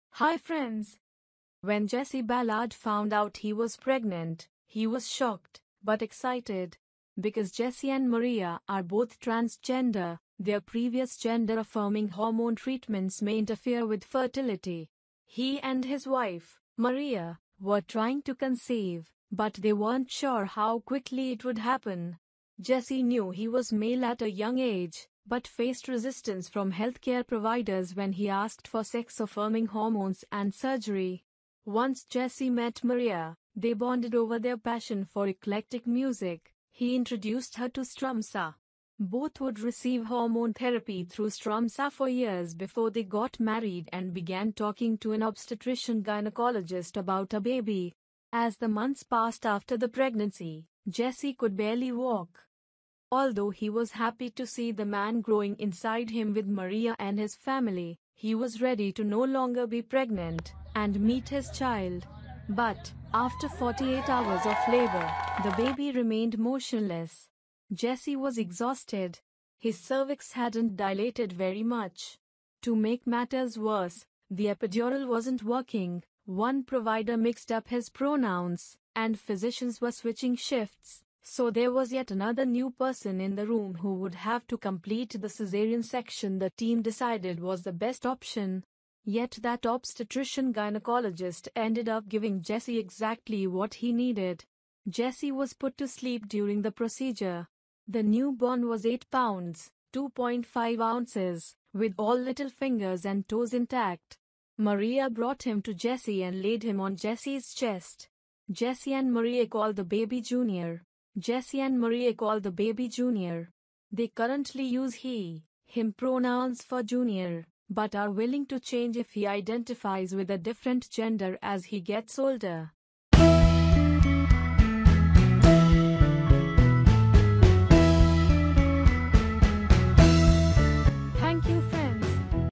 A transgender couple fulfilled their dream - Jesse and Maria Ballard became Natural parents.
Jesse knew he was male at a young age, but faced resistance from health care providers when he asked for sex-affirming hormones and surgery.Once he meet maria and they fall in love.Their dreem to become a parants. This video explains how these beautiful transsexuals, Jesse and Maria, are fulfilling their dreams of becoming parents.